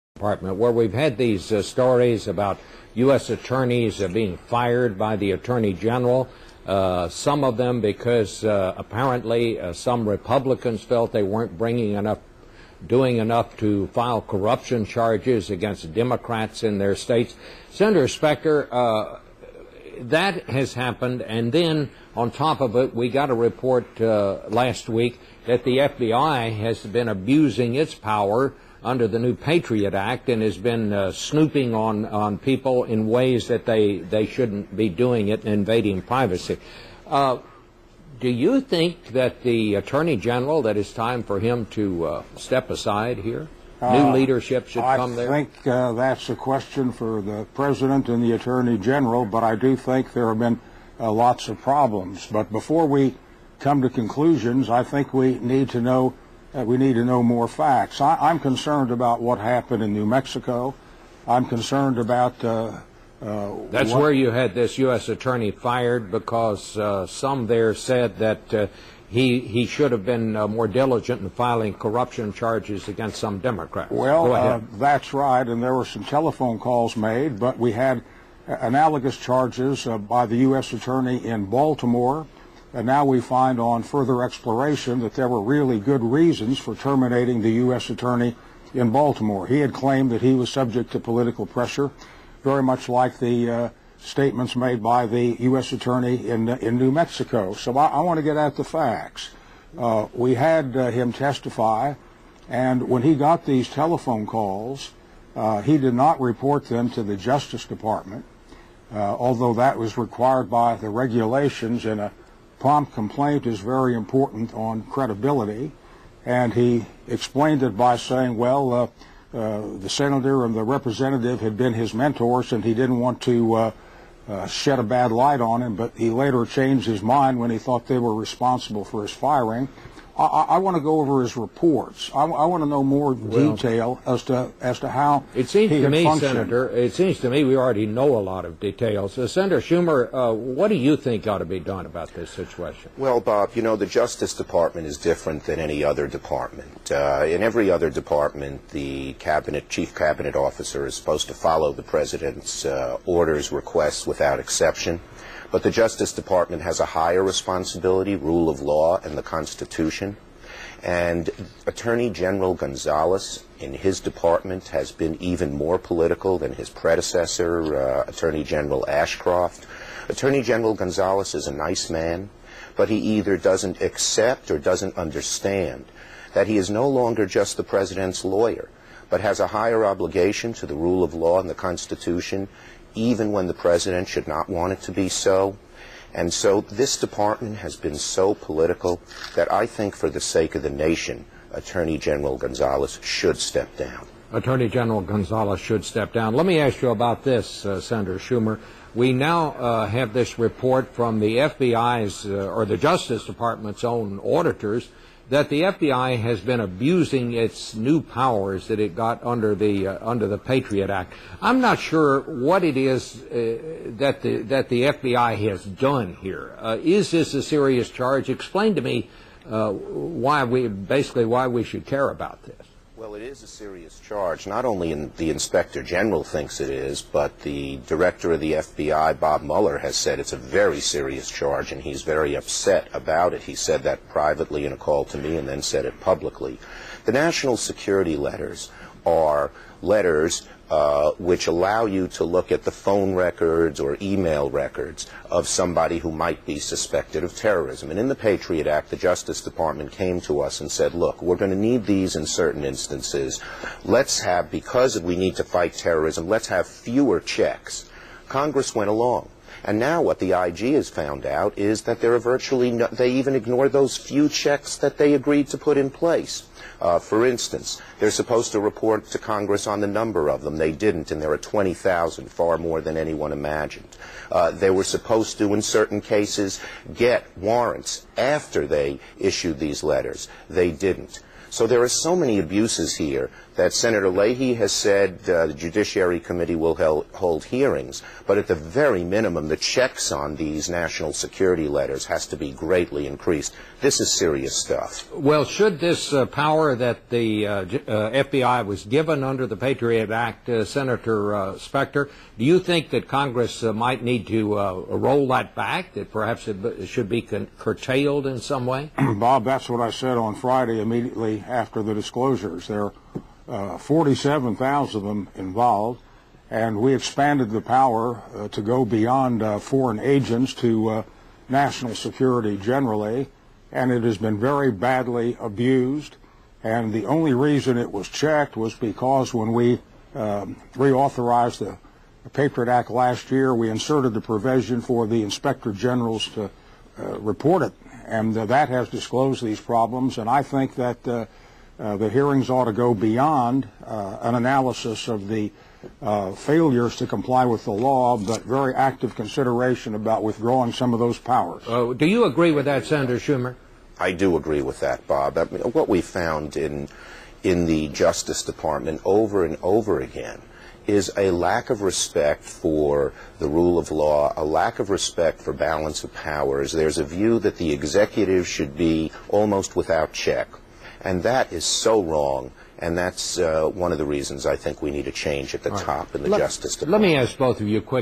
Schumer- Gonzales must go.
Arlen Spector- There are problems at the DOJ.
Schumer Gonzalez should step down over prosecutor firings and FBI privacy violations